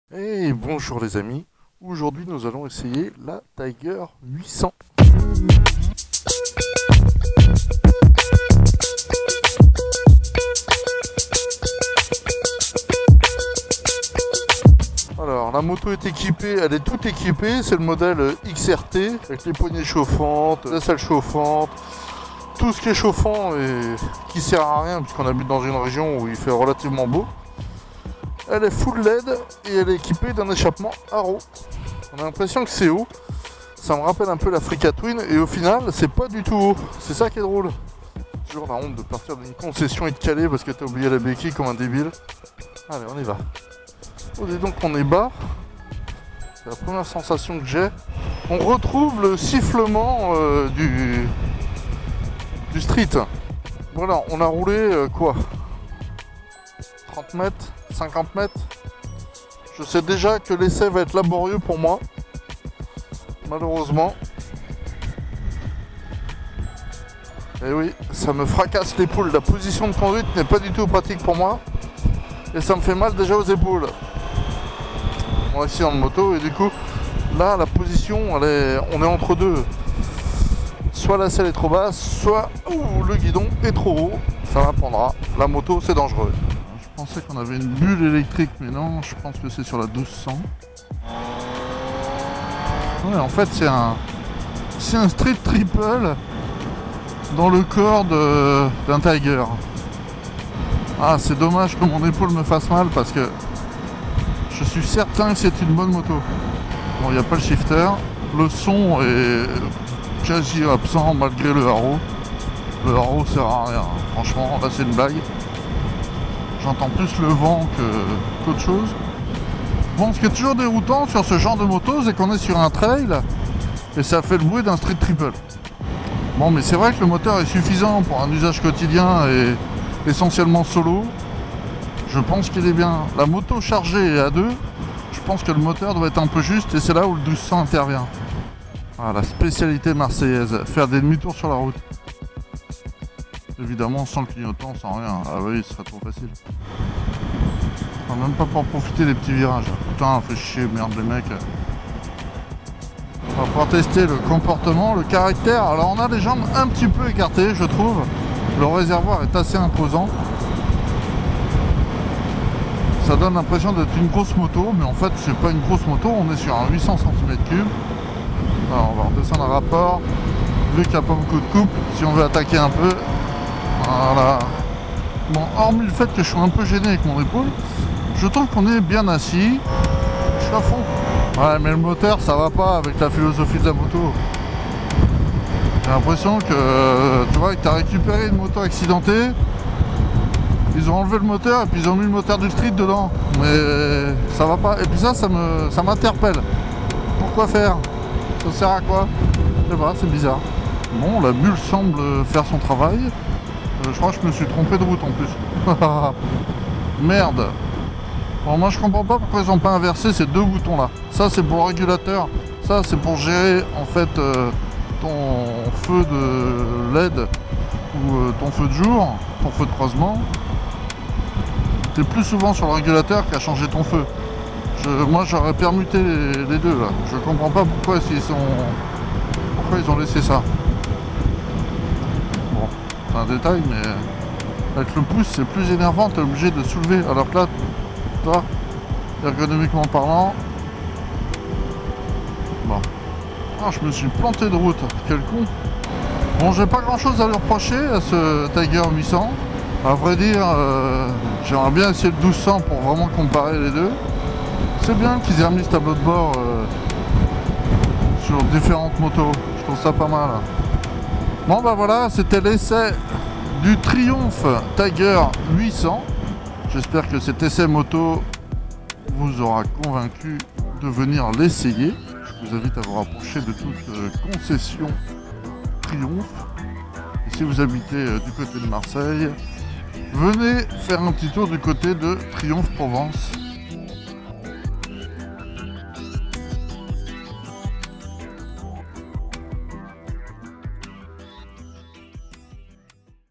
Triumph Tiger 800 - essai moto
Essai moto

Vous prenez le moteur d'un 765 Street Triple, vous reprenez le tableau de bord et vous le glisser dans le cadre d'un trail... voici le Tiger 800... bon, ok, je suis un peu réducteur sur l'approche, mais sincèrement si tu n'aimes pas le Street Triple dans son comportement, tu n'aimeras sans doute pas le Tiger 800...

Il existe également une version pour grande personne, le Tiger 1200 comme on l'appelle aujourd'hui... il est prévu à l'essai, mais pas sur ce coup là, pourtant j'aurais trop aimer les comparer ;-)